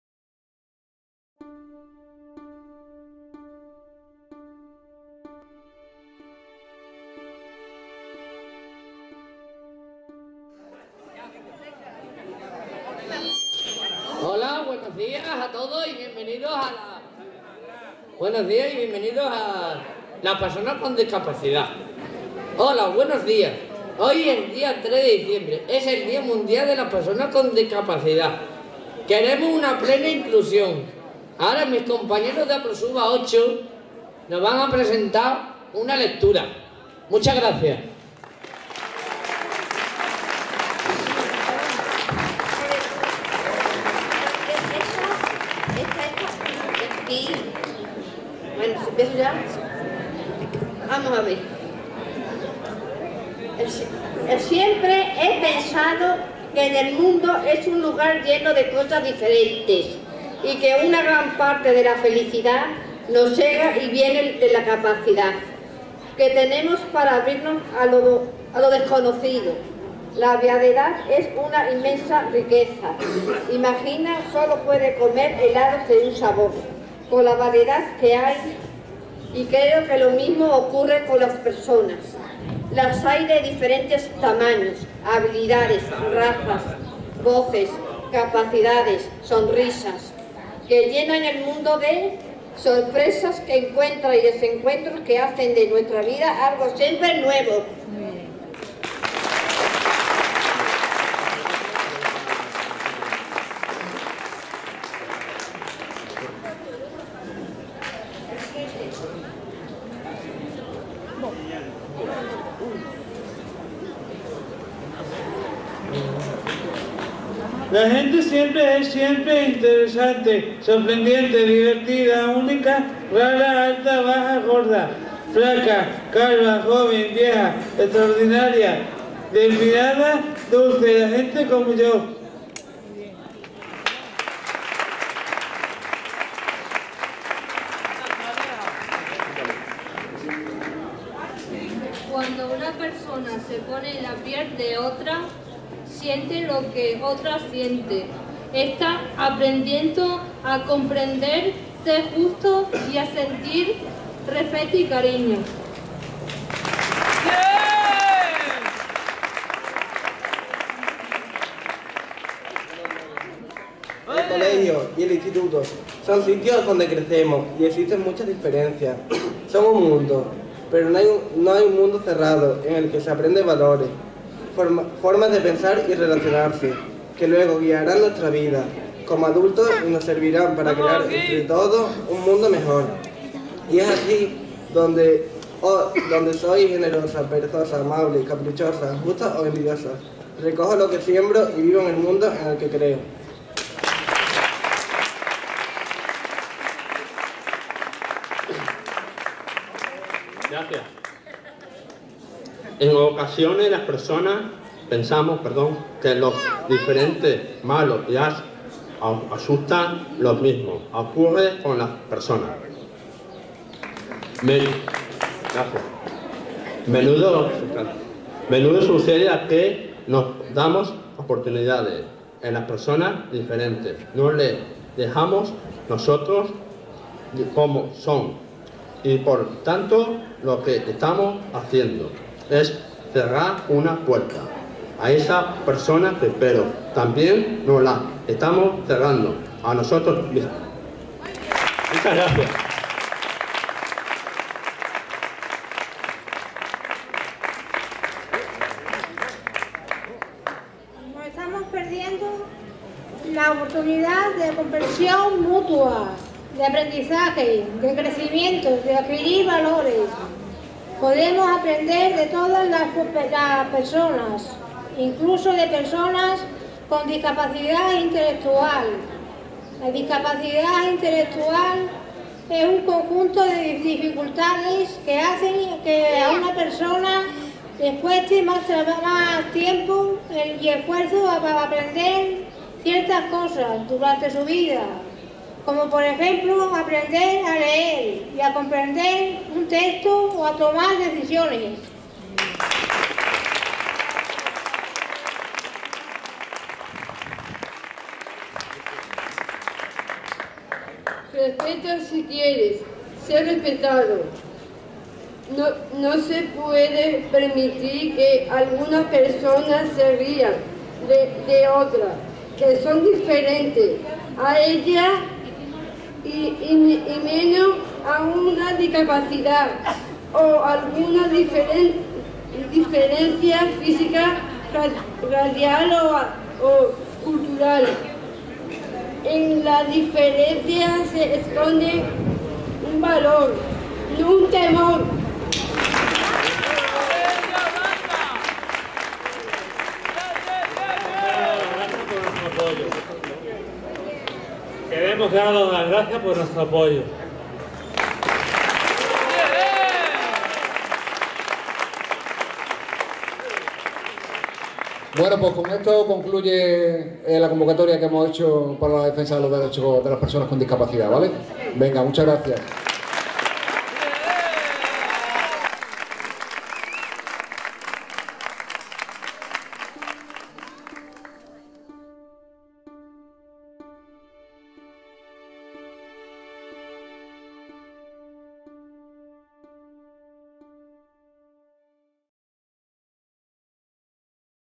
Día de la Discapacidad en Montijo, APROSUBA 8, 3/12/2015
Acto de APROSUBA 8 en Montijo por el Día Internacional de la Discapacidad, para  que la sociedad tome conciencia de las dificultades y barreras que pone a estas personas y que les impide desenvolverse en igualdad de oportunidades en todos los ámbitos sociales, así como para fomentar el respeto a la diversidad.

3 de diciembre de 2015